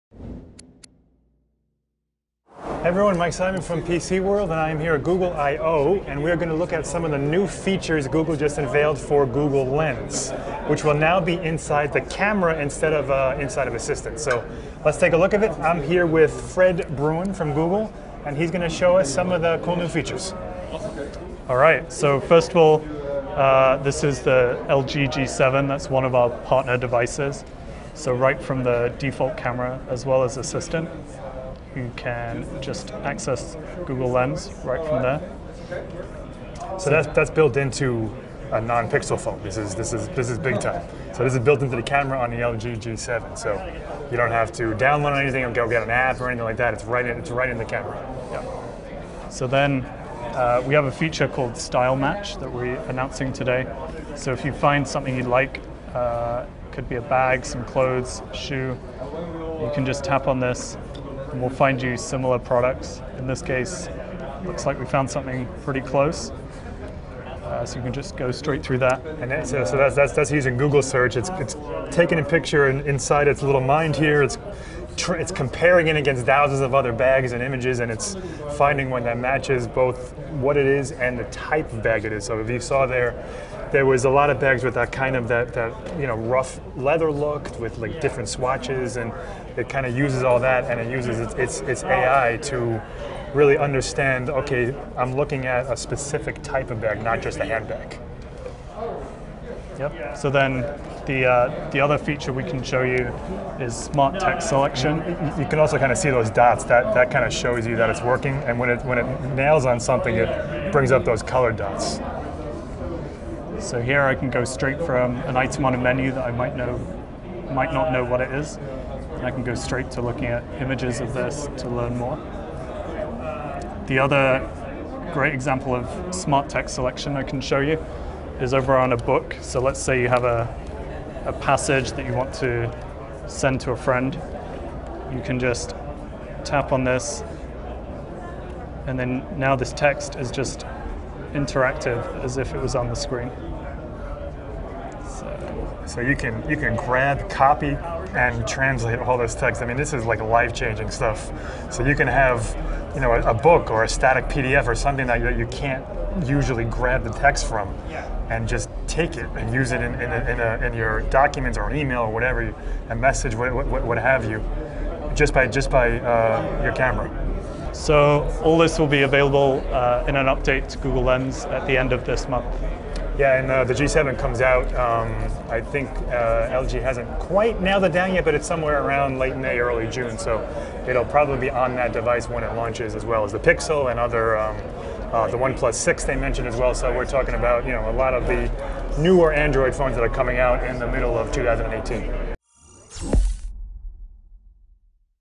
IDG.tv | 새로운 구글 렌즈 사용기 [한글 자막]
구글 렌즈(Google Lens)는 이제 안드로이드 카메라 앱의 일부이며 몇 가지 멋지고 새로운 기능을 제공합니다.